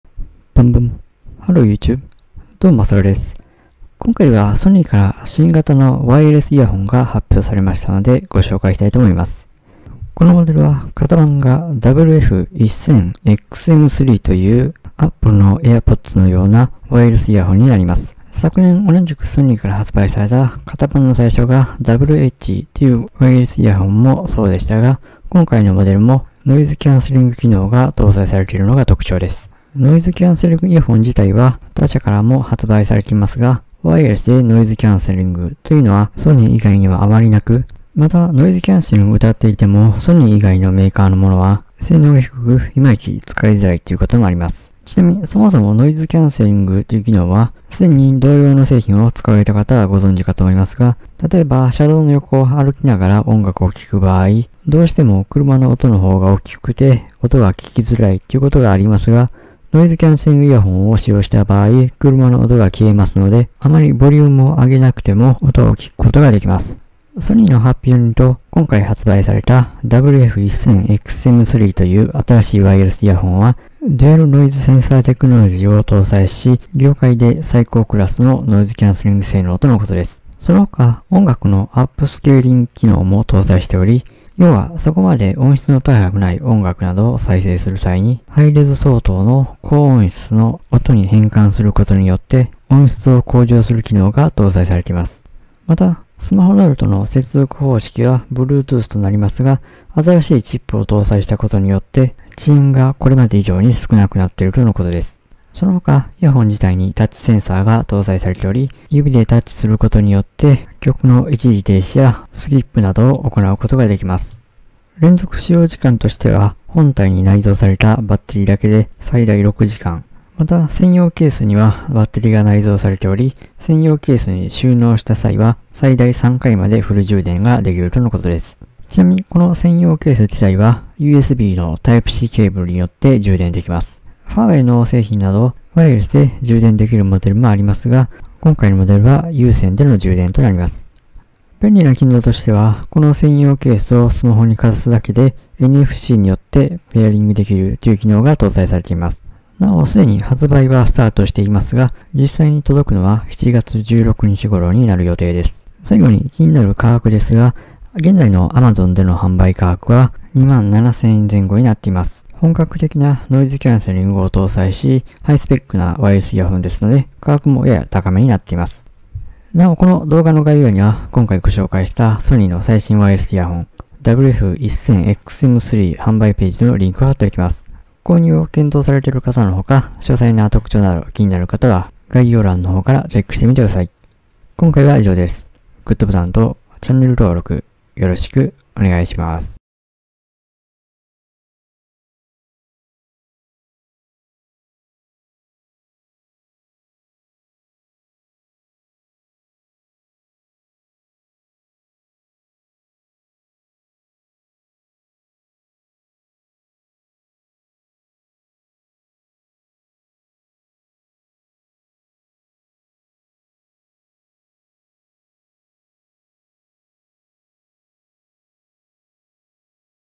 ノイズキャンセリング搭載 ソニー新型ワイヤレス イヤホン WF－1000XM3 発売開始
ソニーの新型【ワイヤレス】イヤホン WF-1000XM3 が発売されました。世界最高のノイズキャンセリング機能を搭載した WF-1000XM3 の特徴や価格をご紹介します。

▼ソニー 新型イヤホン WF-1000XM3（ブラック）

▼ソニー 新型イヤホン WF-1000XM3（シルバー）

▼子供に夢をたくすな！

無料体験 も用意されています

▼ビジネス書からライトノベルまで 本を聴くサービス